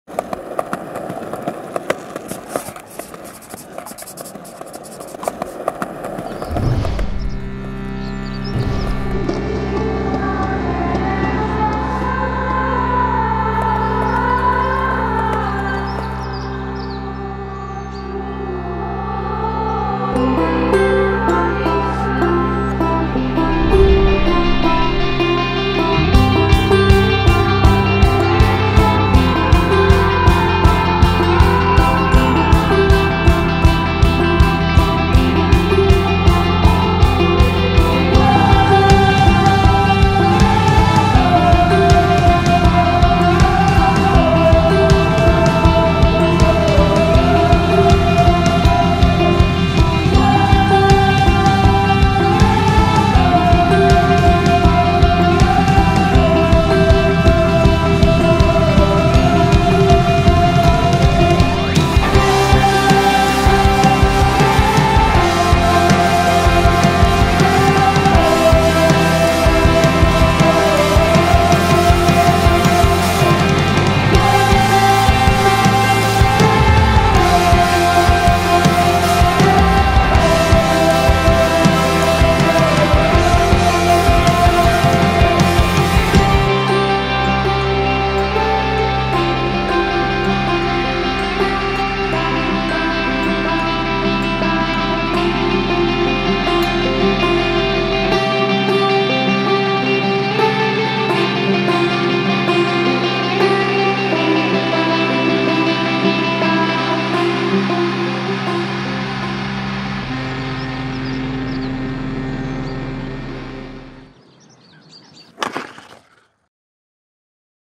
Penny Australia x Christian Hosoi Signature Model
Penny Skateboards is proud to introduce the first of their signature models with Christian Hosoi.
Known for his raw and dynamic approach to skateboarding, Christian Hosoi brings his legendary graphic to Penny Skateboards. Featuring an epic graphic wrap and custom transparent griptape, the Hosoi Signature model is a must have for your Penny collection.

Read more about the Penny x Christian Hosoi collaboration on the Penny blog
Filmed and Edited by Ryan Spencer